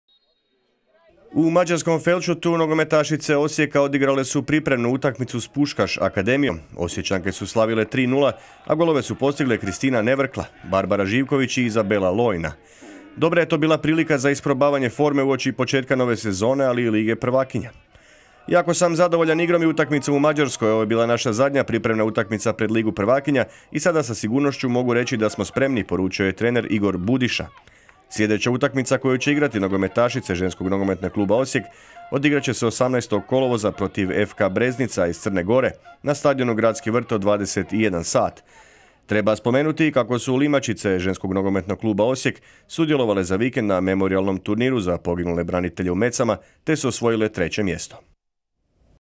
Osječanke bolje od Mađarica iz Puskas Akademije u pripremnoj utakmici
Nogometašice ŽNK Osijeka odigrale su prijateljsku utakmicu s mađarskim prvoligašem Puškaš Akademijom. Zadnja je to provjera prije Lige prvakinja, a Osječanke su slavile 3:0.